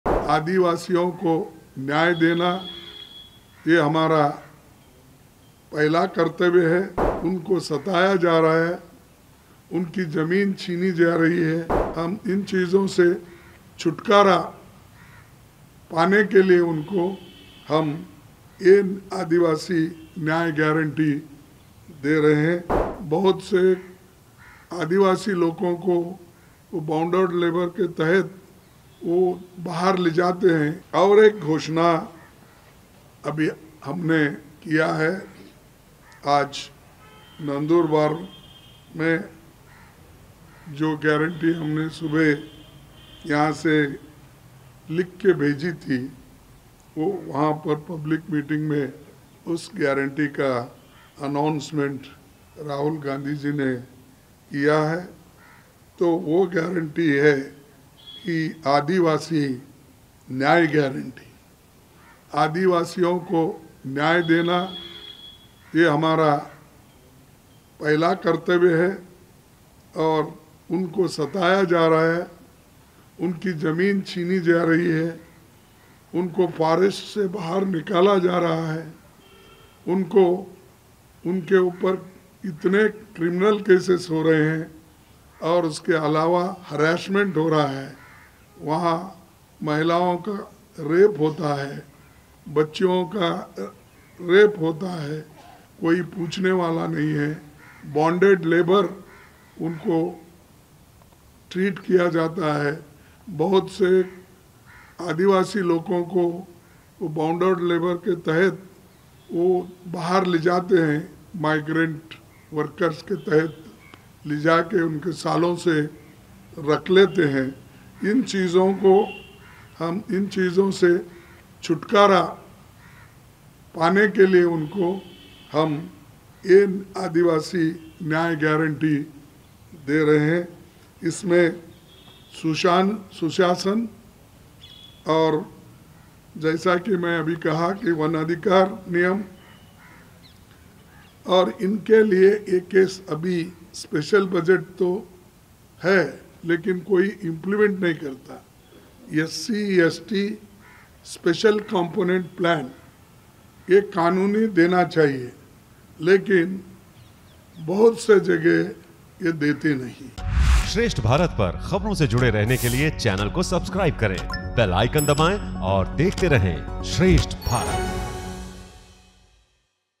आदिवासियों को न्याय को लेकर Mallikarjun Kharge का ऐलान | Adivasi Nyay | Congress | Election 2024
Congress के राष्ट्रीय अध्यक्ष Mallikarjun Khargeने कहा, "आदिवासियों को न्याय देना हमारा पहला कर्तव्य है। उन्हें सताया जा रहा है। उनकी जमीन छीनी जा रही है उनके साथ बंधुआ मजदूरों की तरह व्यवहार किया जाता है हम इन सभी चीजों से छुटकारा पाने के लिए 'आदिवासी न्याय गारंटी' दे रहे हैं